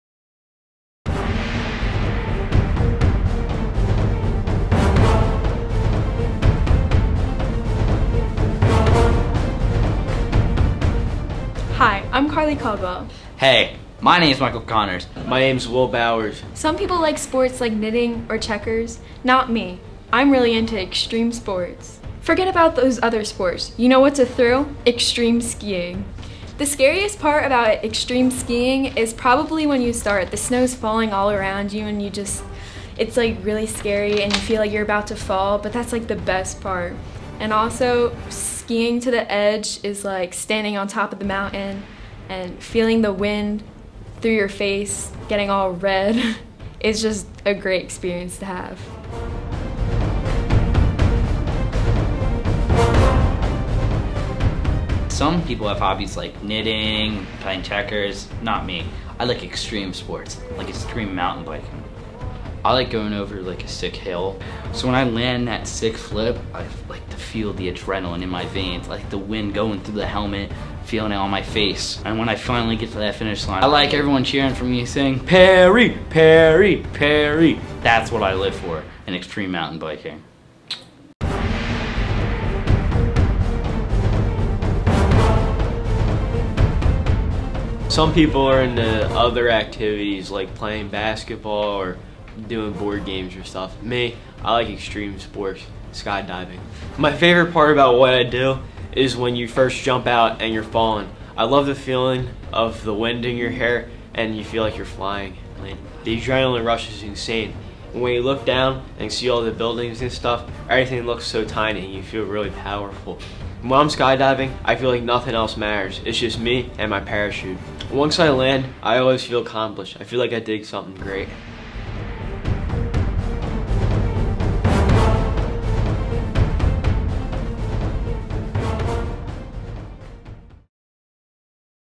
Extreme Sports Gabriella Caputi
1. Making a documentary is different from making a regular scripted movie because more work goes into editing than filming. While filming simply calls for people sitting in front of a green screen and saying a few lines at a time, the editing is where the b-roll makes the movie a movie. The mechanics of editing a documentary are more difficult than a regular movie.
2. While shooting the interviews it is definitely easier than traveling to different locations to shoot and having more than one person in a shot, but shooting requires actors to know more than one line at a time well, and lengthier individual lines. The acting needs to be natural, like the actors are real people.
3. When editing the documentary, there was a lot more stuff that needed to be layered and refined than usual. Filters and b-roll needed to be configured smoothly.
4. I like making a regular scripted movie better because its's overall more interesting to film than a documentary. It also requires more creativity instead of simply placing b-roll over jump shots and to provide visuals to the viewer.